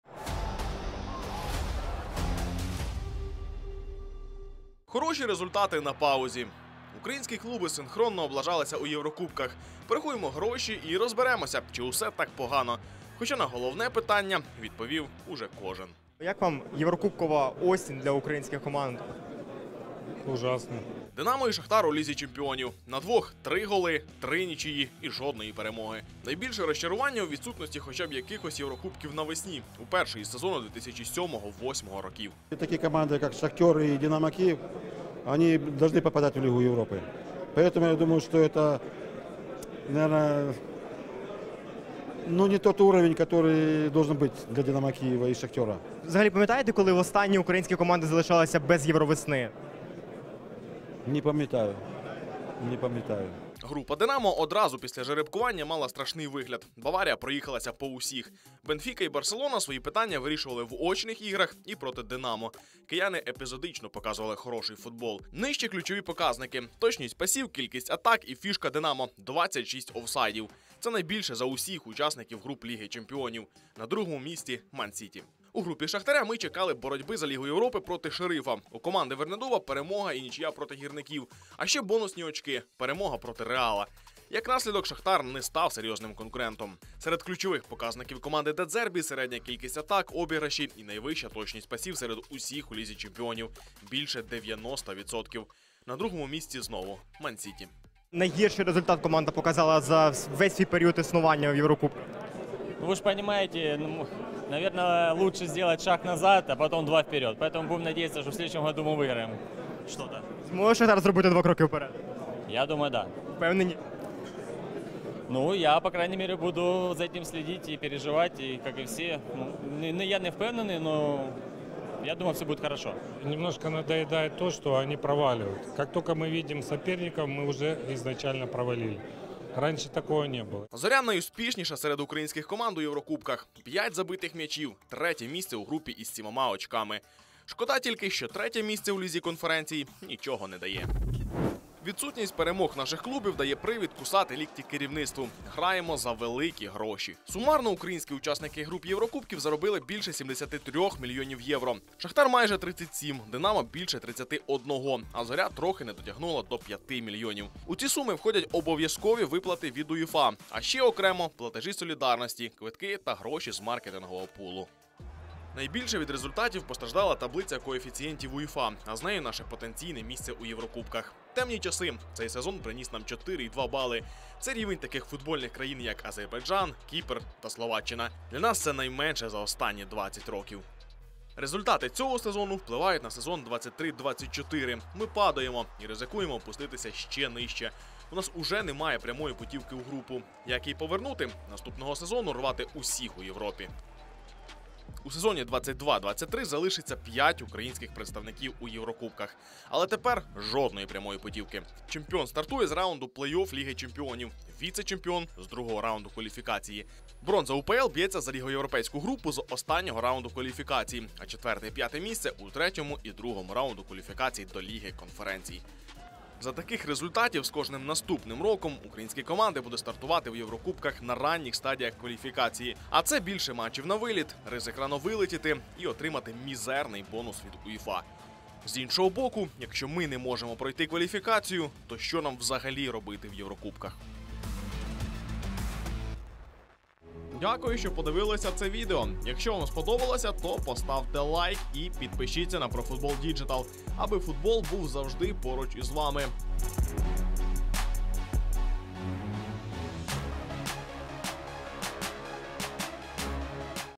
МІЛЬЙОНИ для ДИНАМО, ШАХТАРЯ і ЗОРІ 🤑 Цифри Ліги чемпіонів і Ліги конференцій. Чекаємо трансферів?
Скільки заробили українські клуби за цей євросезон у Лізі чемпіонів і Лізі конференцій? Відомі головні цифри Шахтаря, Динамо і Зорі за участь у єврокубках сезону 2021/22: статистика, гроші та бали у таблицю коефіцієнтів УЄФА. Скільки наших команд буде у наступному євросезоні і як вони там гратимуть - про це у сюжеті.